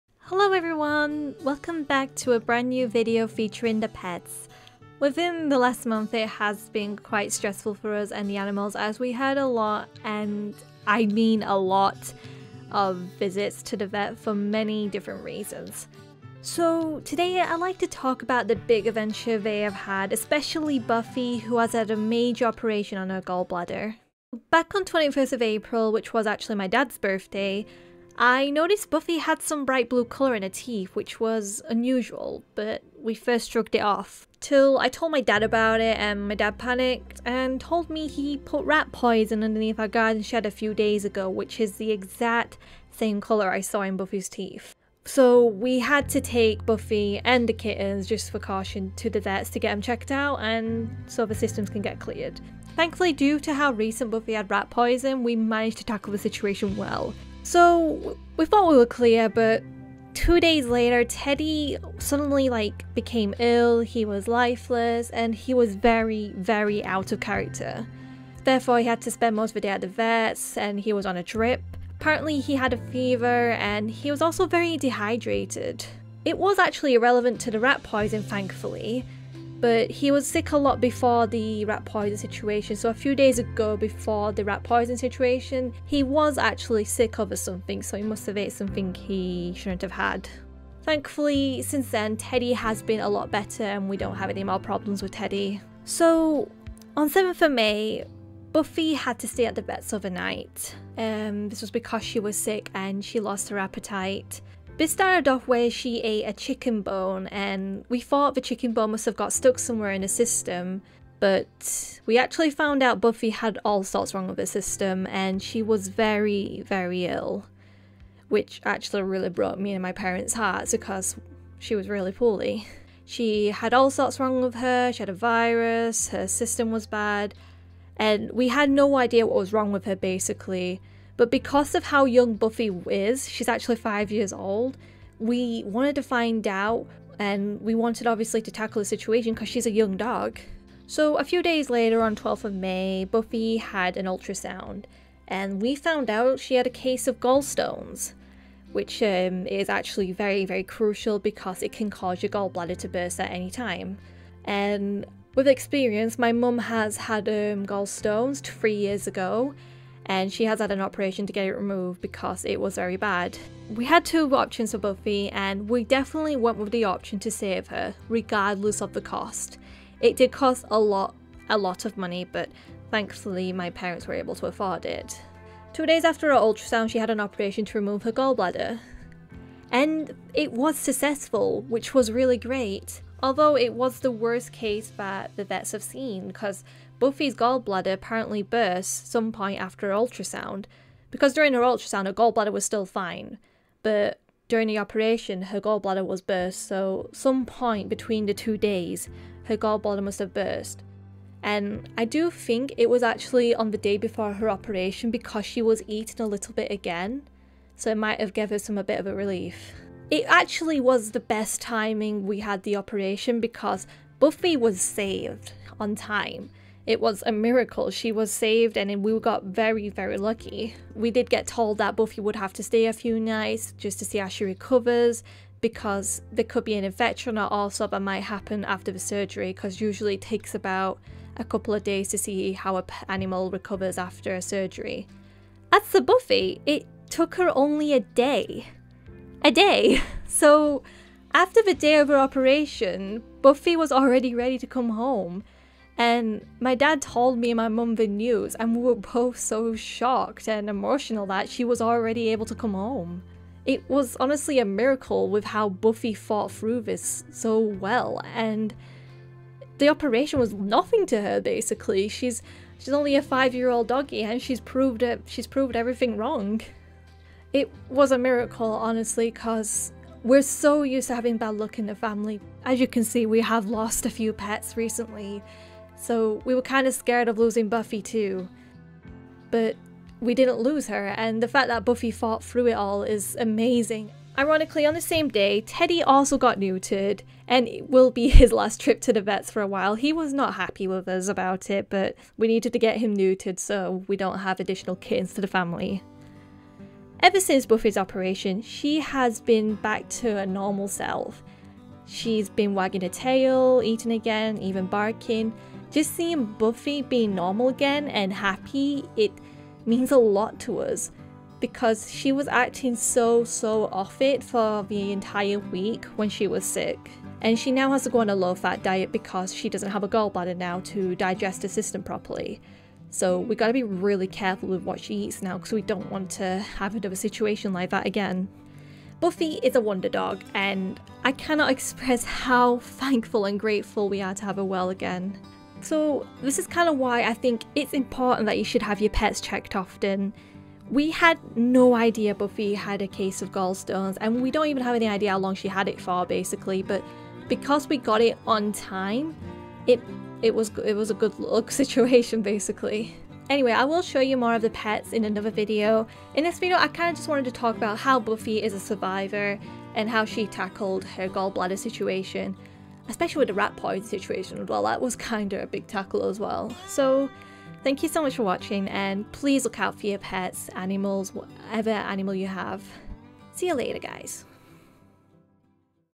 My Sheltie With Gallstones
Hiya all! It's been a hectic month with many vet visits but we've finally made it. I wanted to share Buffy's story of how she survived gallstones and how extremely well she processed through the week. With some additional stories of all the pets going to the vets due to a minor rat poison situation (which Buffy had some tablets due to it, wasn't mentioned in the video), Teddy going back to the vets due to being ill and getting neatured.

Buffy is a wonder dog and it amazes us how well she recovered after her surgery. You can always find out about my pets on their instagram

💌 Any extra support is appreciated!